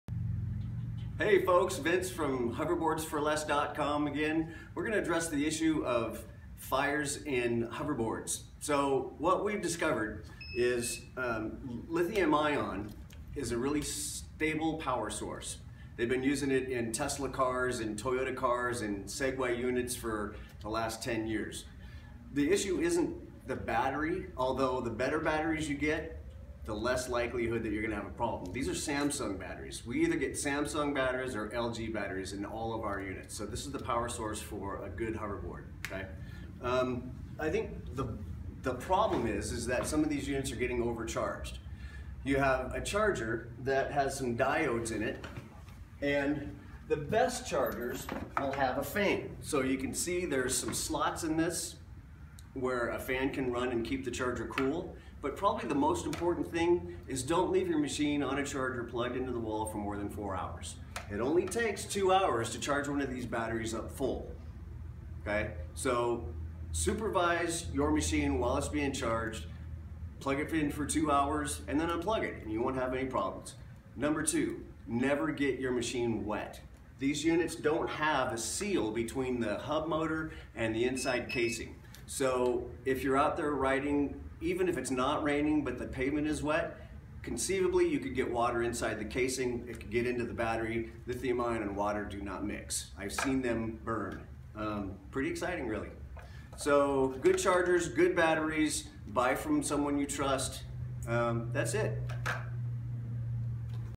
Hoverboard Battery Safety - Are Hoverboard Batteries Safe
- Hoverboards are in the news for many reasons since they are the hottest thing around. A lot of media hype is playing about the safety of hoverboards and there have been reports of units catching on fire. Watch this video to learn why there is more hype than reality and how to avoid issues by purchasing good quality units with the better bateries and charges. We also will talk about how to charge for safety. HINT: Never leave them plugged in longer than 4 hours - 2 hours is typically good. And do not get these units wet, they are not made for water.